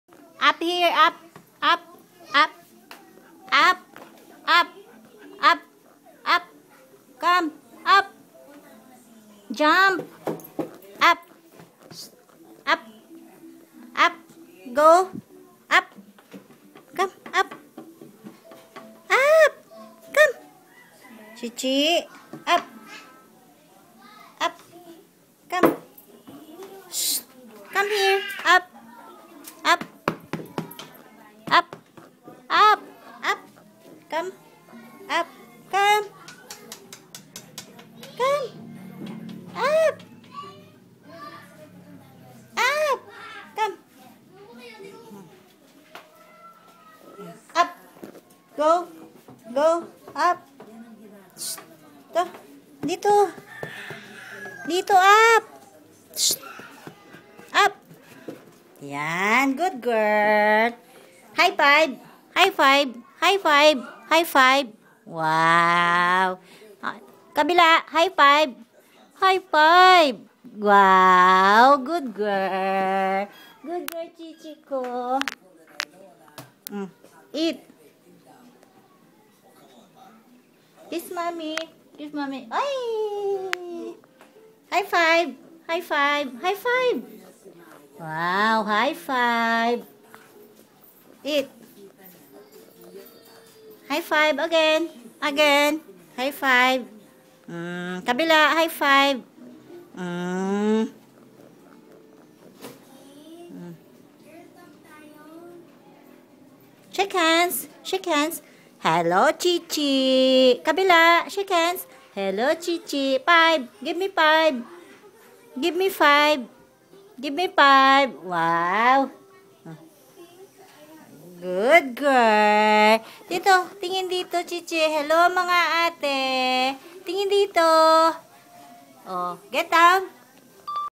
Chieh Chieh Bear and Mom:) Happy 4mons old! 011614
At four mons... chieh chieh learned tricks like  sit, come, shake hands and high five... We're so excited what more tricks she will learn...thanks kuya and mom for the patience haha :)